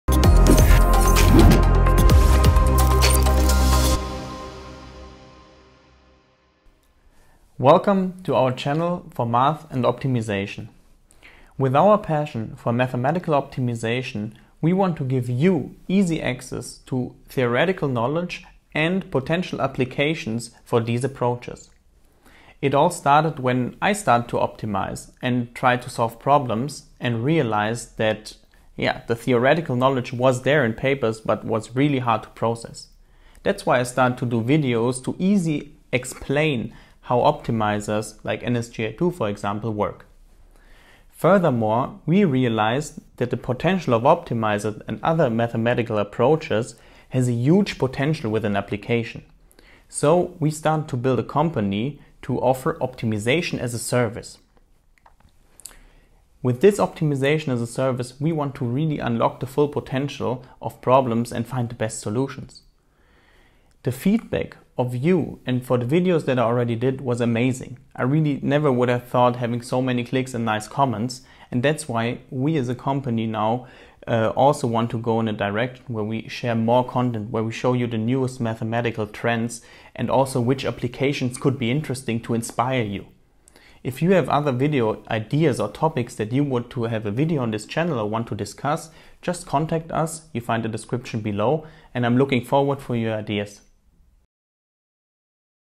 Welcome to our Channel - Optimization Geeks
Hello and welcome at Optimization Geeks,

with our passion for mathematical Optimization, we want to give you easy access to theoretical knowledge and potential applications for these approaches.

It all started when I started to optimize and realized that the theoretical knowledge was there in papers but was hard to process. That’s why I started to do videos to easily explain how optimizers work (i.e. NGSA-II). Furthermore, we realized that the potential of optimizers and other mathematical approaches has a huge potential within an application.

So we started to build a company to offer Optimization as a Service. With Optimization as a Service, we want to Unlock the full potential of problems and find the best solutions.

Your feedback on our last videos motivates us to share more content and show you the newest mathematical trends and also which application could be interesting to inspire you.